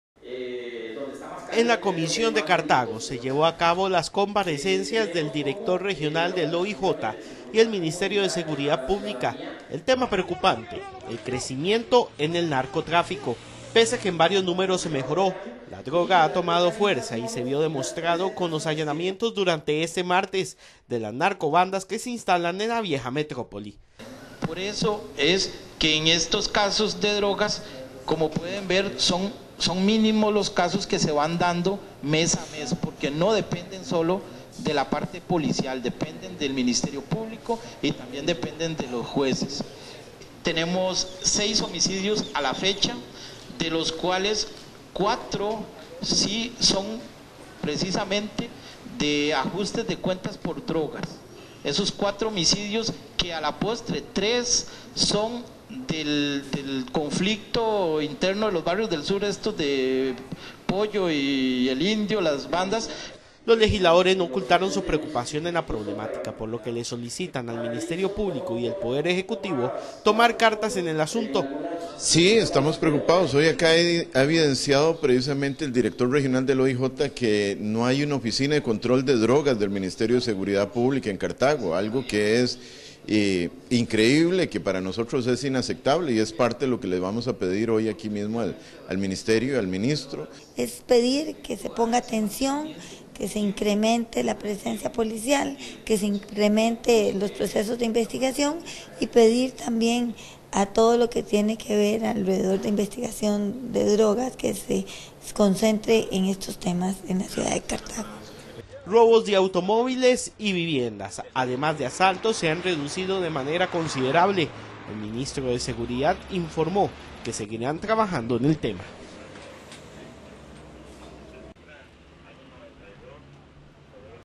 CRECIMIENTO DE NARCOTRÁFICO EN CARTAGO PREOCUPA A DIPUTADOS
Pese a que se ha mejorado muchas cosas en materia de seguridad en Cartago. El narcotráfico toma poder y el OIJ, no oculta su preocupación por el poco personal para atender el problema.